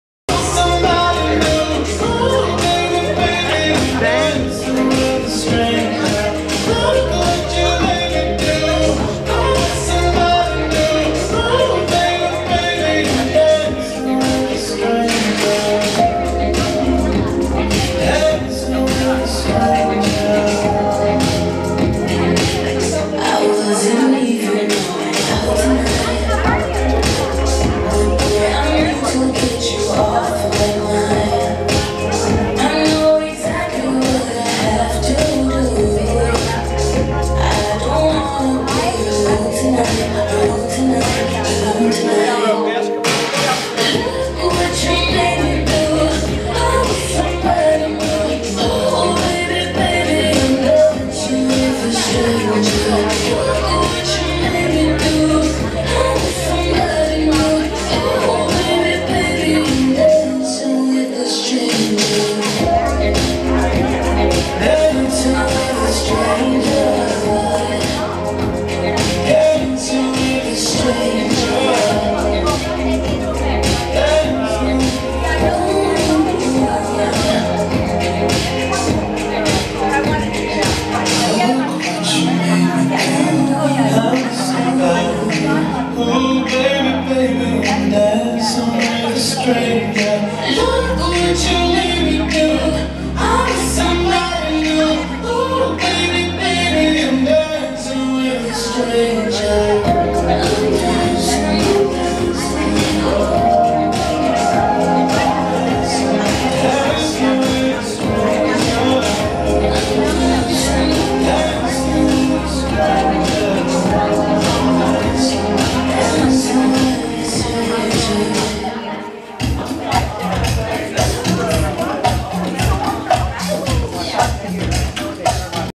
Ben hustle dancing with Erica Smith at VFW post in PA.
Ben hustle dancing with Erica Smith for the first time 4/13/19